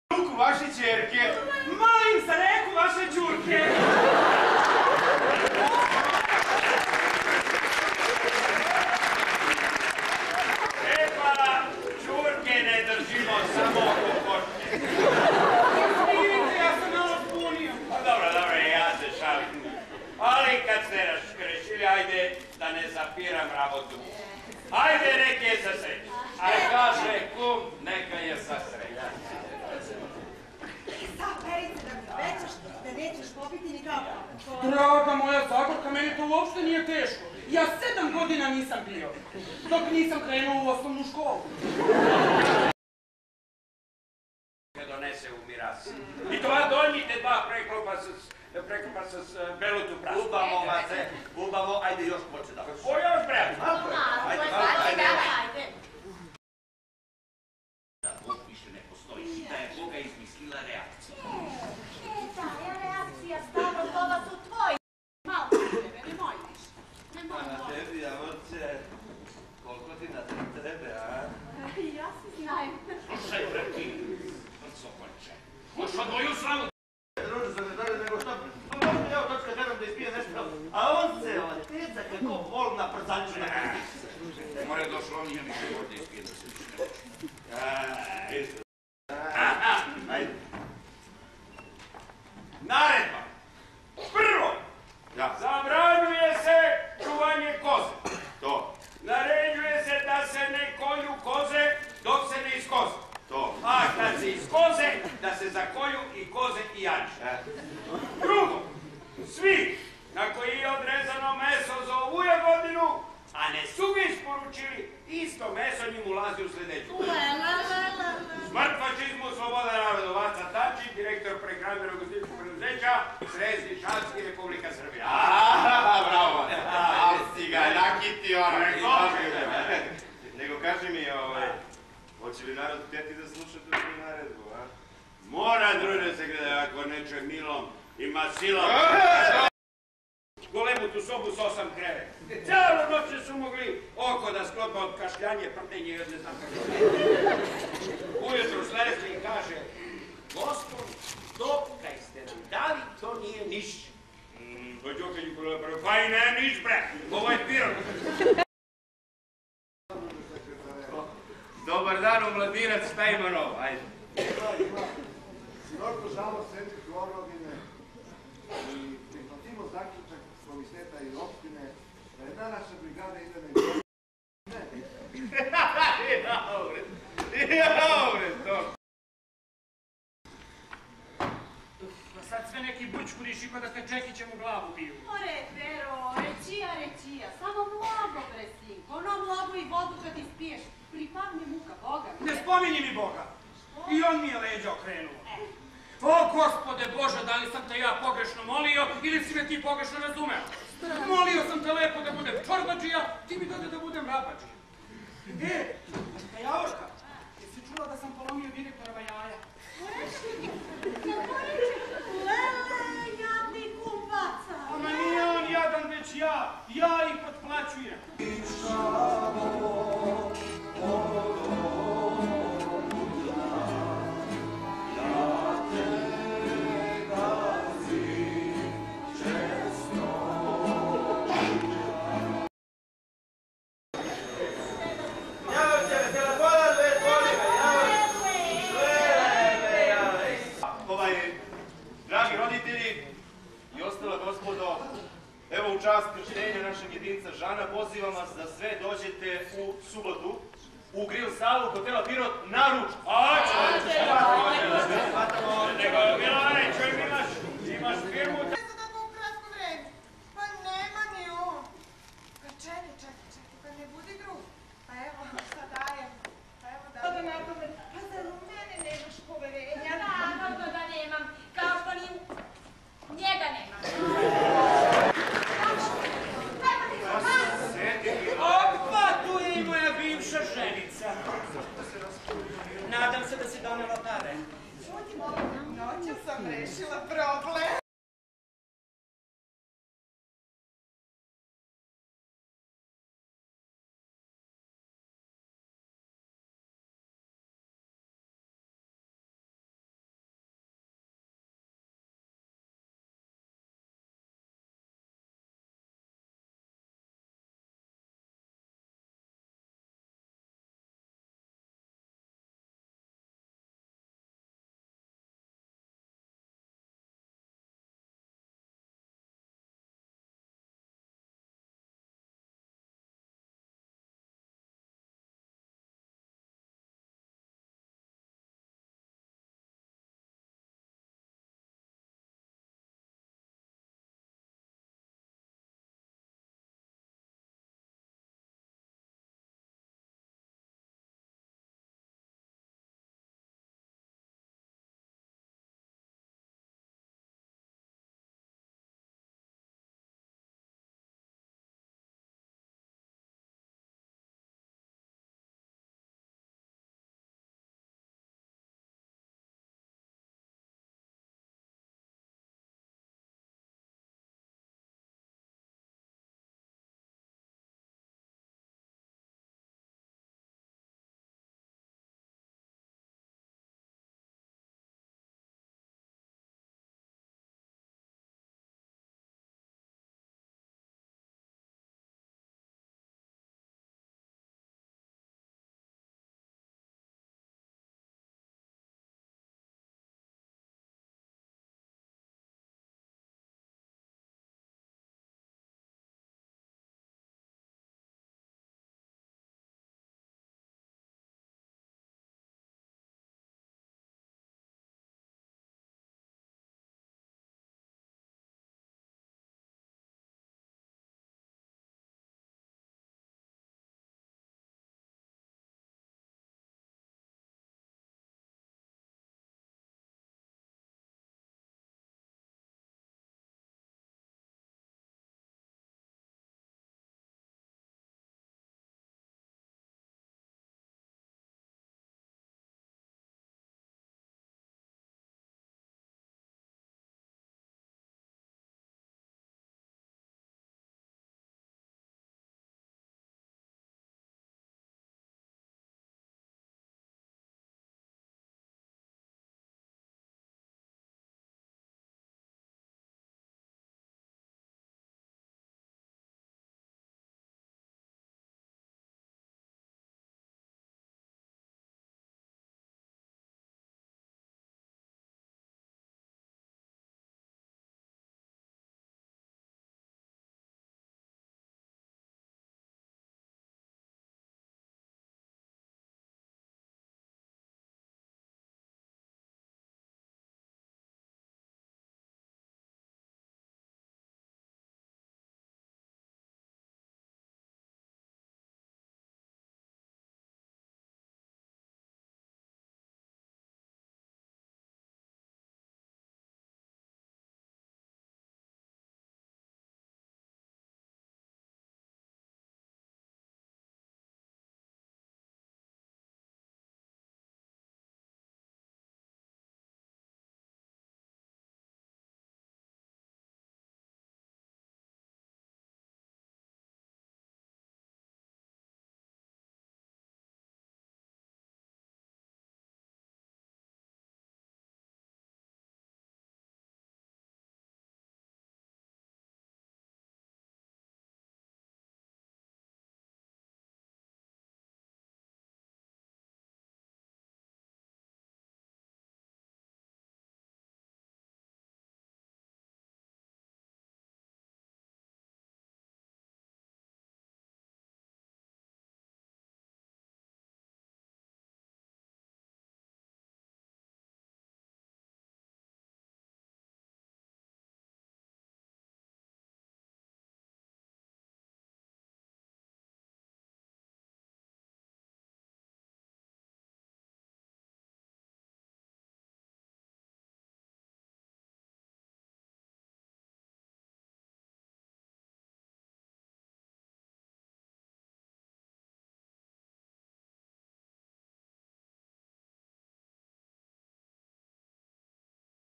How to make costumes for theater plays PART I
I made costume designes for this theater play

predstava A Nisava tece Narodno pozoriste Pirot
kostimi Jelena Zlatkovic Velickovic